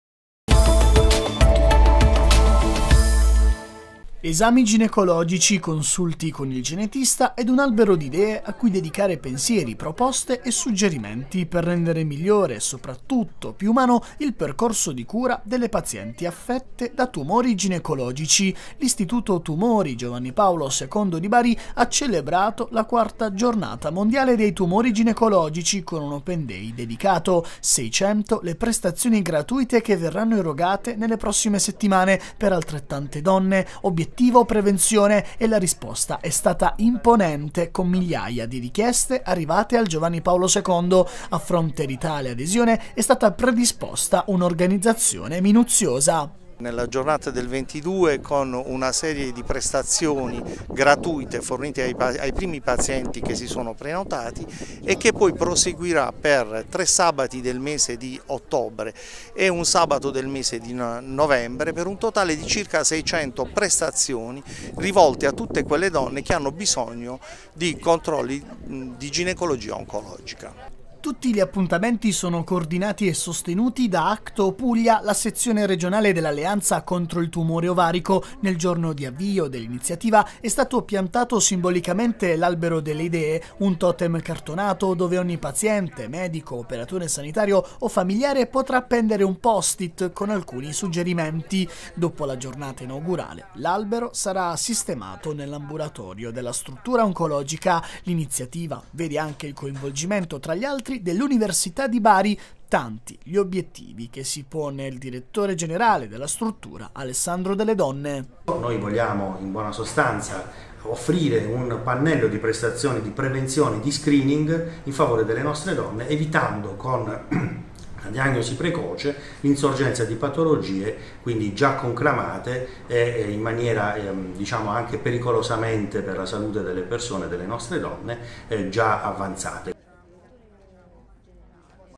BARI | Open day tumori ginecologici, visite e consulti per 600 donne a Bari
Iniziativa dell’Istituto Tumori “Giovanni Paolo II”